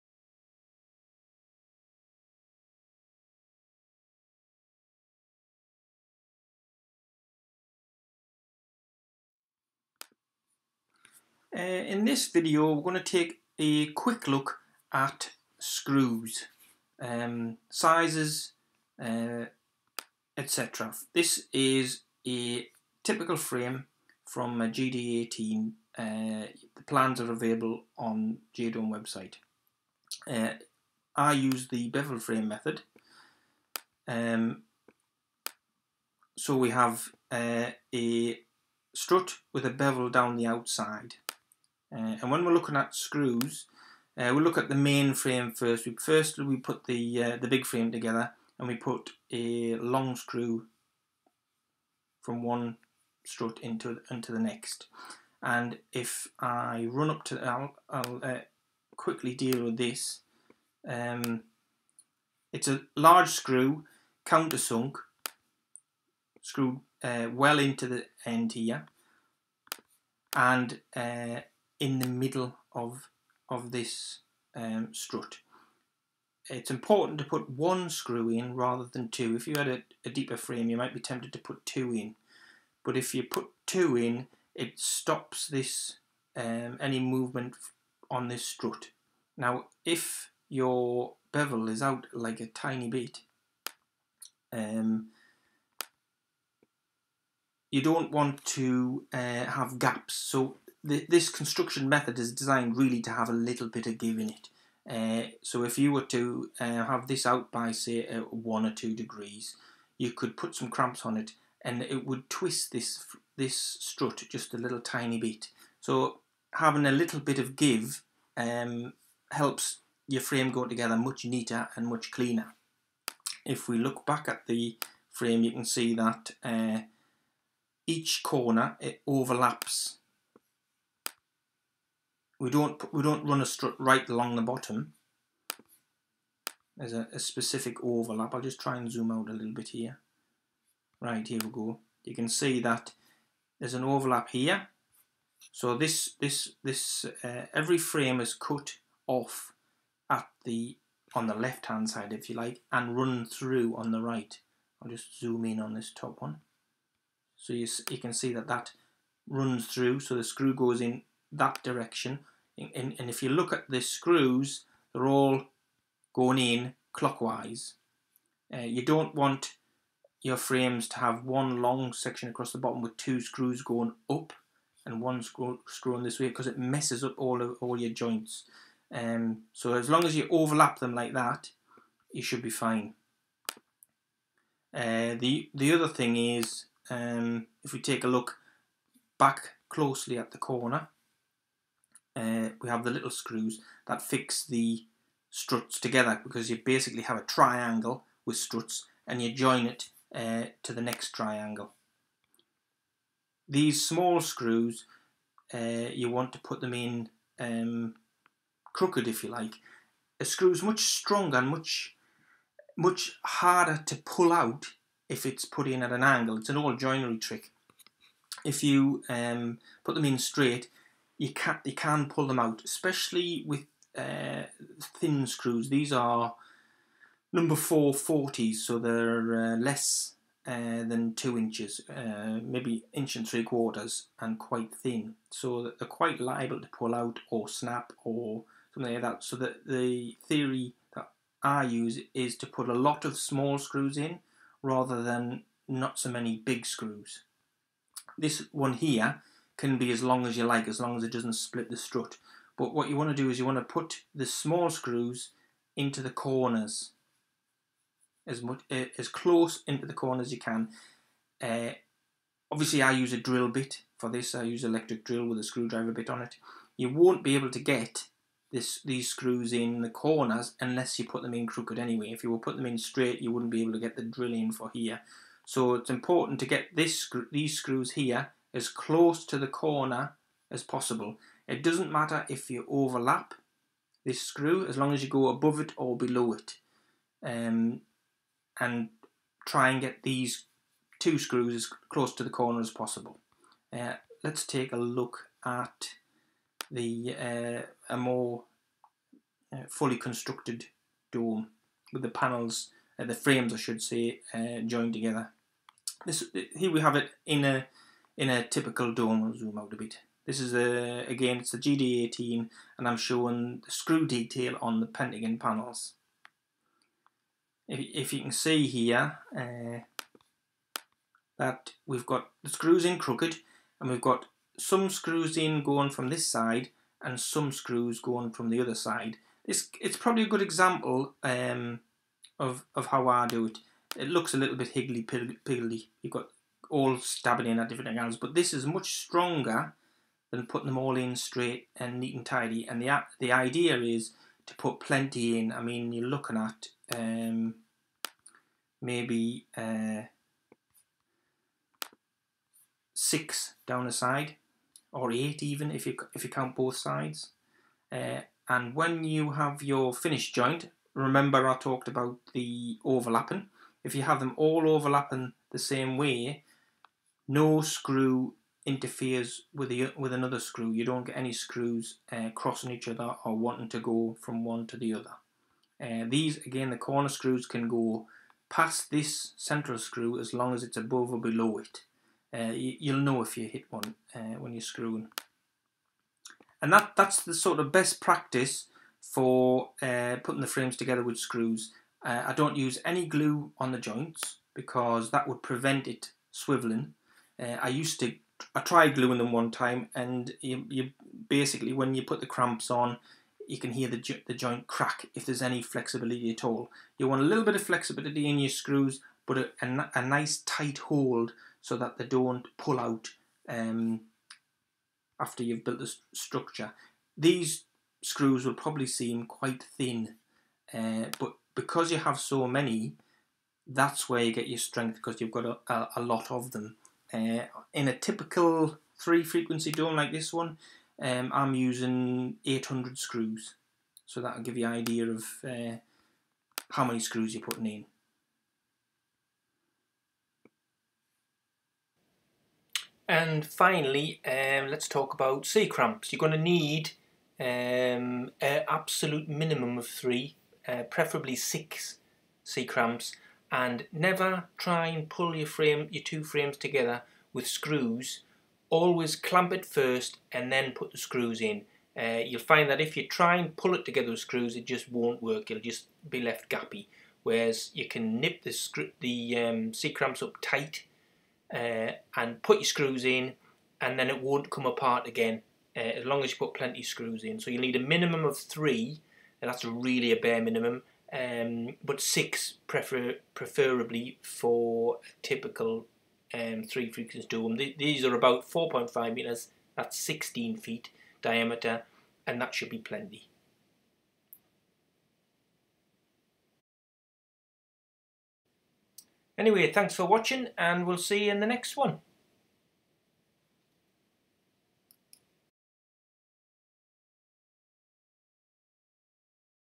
Screwing geo-dome frames together
Just a quick video with some tips and tricks when screwing geodesic dome frames together. I use the beveled frame method, this may be useful for anyone who has purchased plans and wants more info on how I do it.